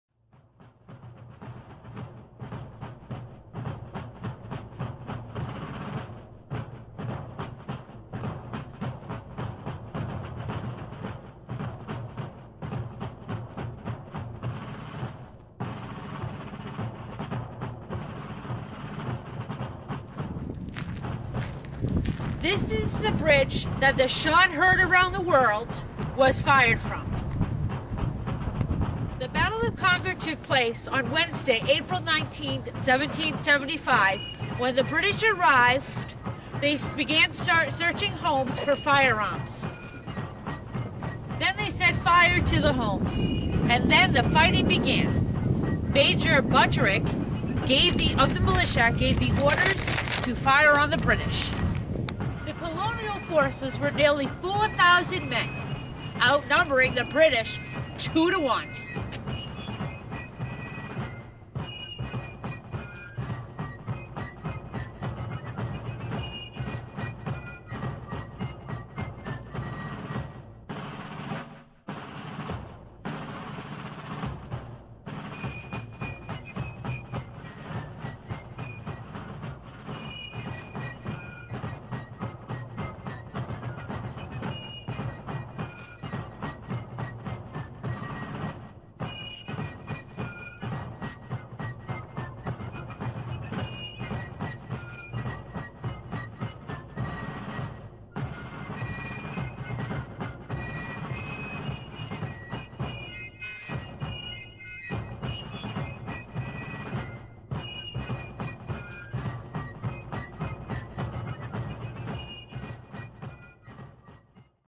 The shot heard round the world
"The shot heard round the world" is the term coined for the opening shot of the Battle of Concord in 1775, the starting point of the American Revolutionary War. The Battles of Lexington and Concord were the very first military engagements of the Revolutionary War. These were fought on April 19, 1775.

Early on April 19, 1775, 700  British soldiers were marching toward Concord, there they fought a brief skirmish with militiamen on Lexington Green, leaving eight colonists dead and nine wounded.